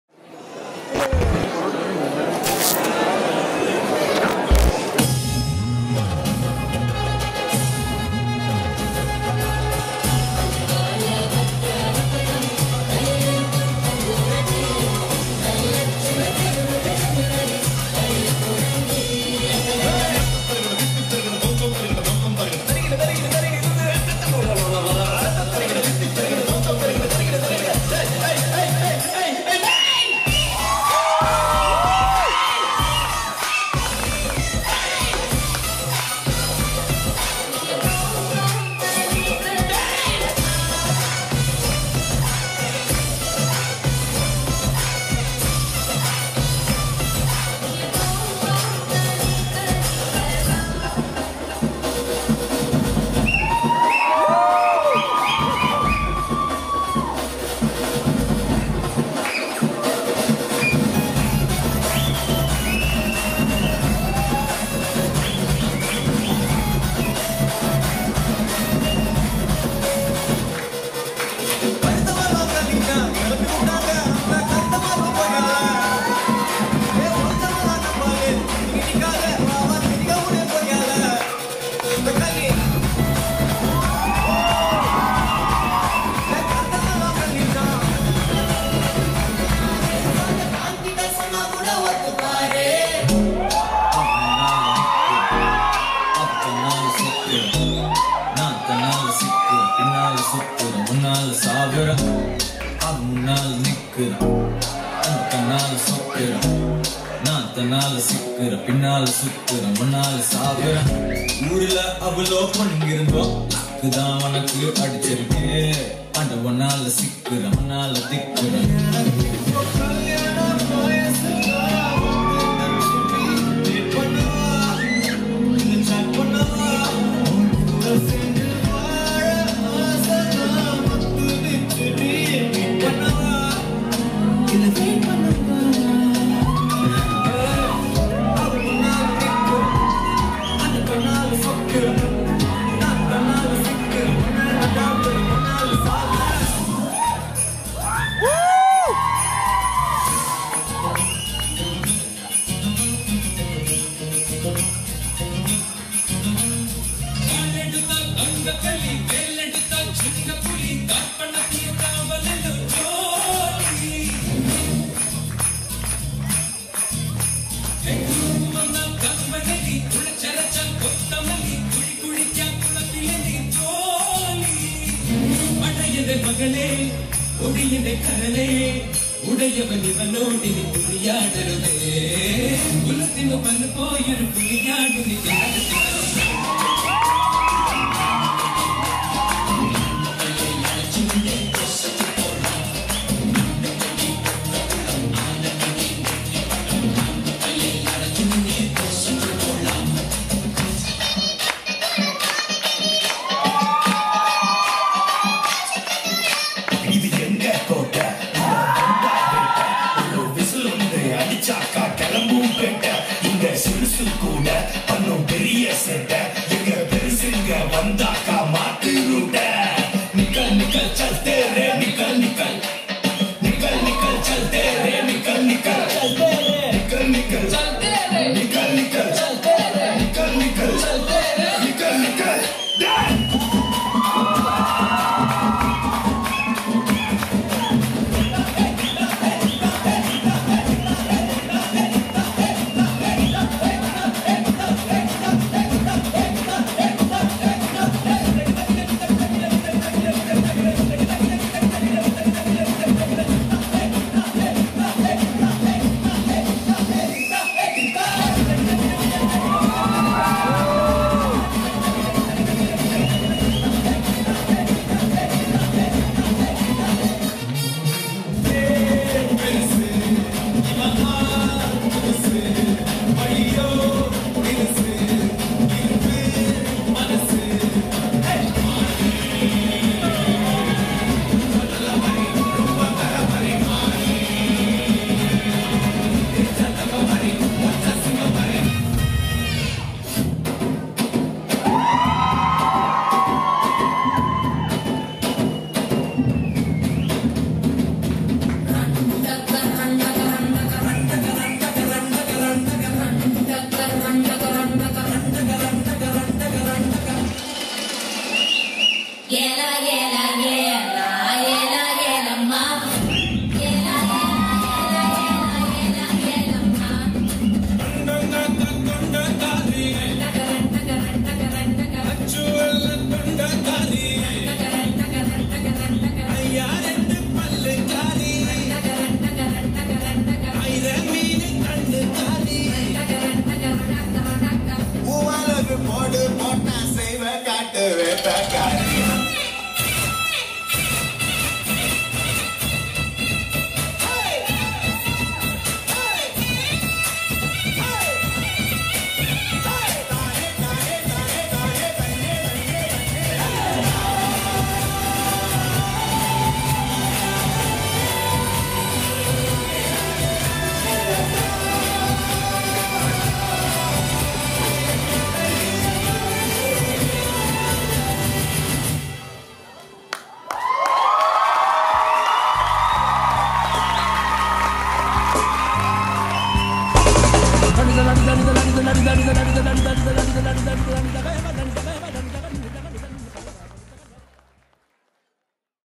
Semma Locals | Fine Arts India Vienna Onam 2019 | Sylvia Kailath x Bluinse Thomas x Stefy Srambical
Dancers:
Sylvia Kailath
Stefy Srambical
Bluinse Thomas
Jenno Thanikkal

Song Editing:
Edwin Parayampillil